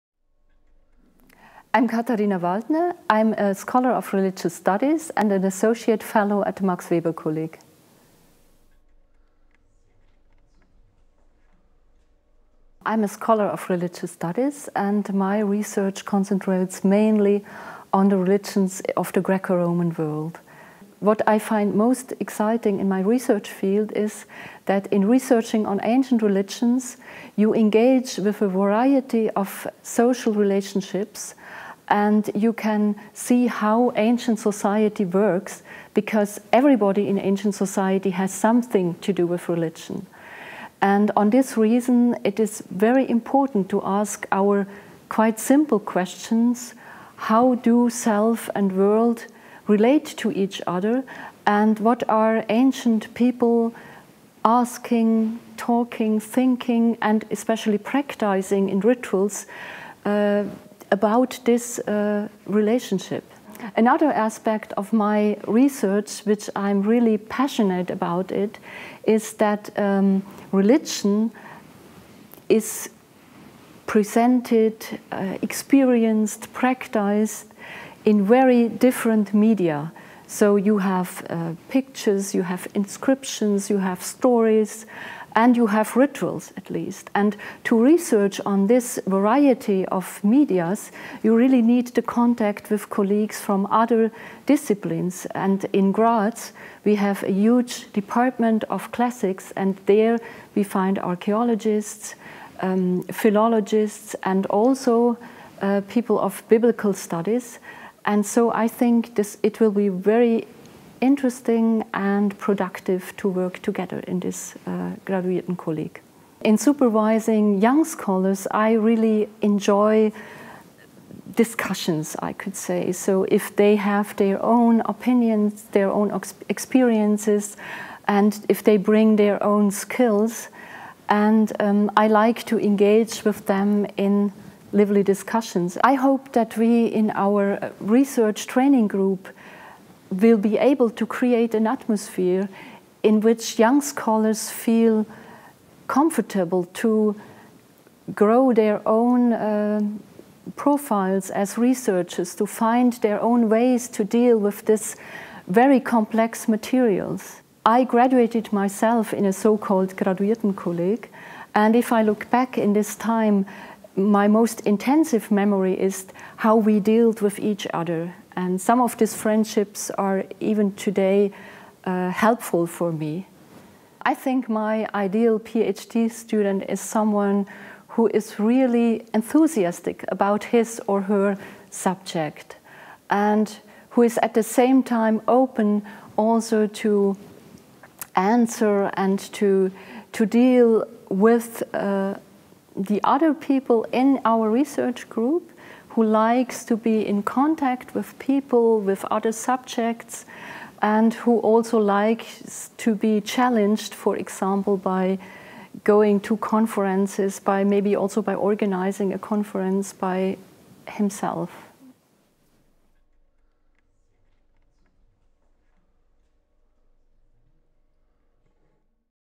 Research Portrait Katharina Waldner (University of Erfurt)
Prof. Dr. Katharaina Waldner
Professor of Religious Studies (University of Erfurt)
Research Interests: Religious Studies in Mediterranean Antiquity, Early Christianity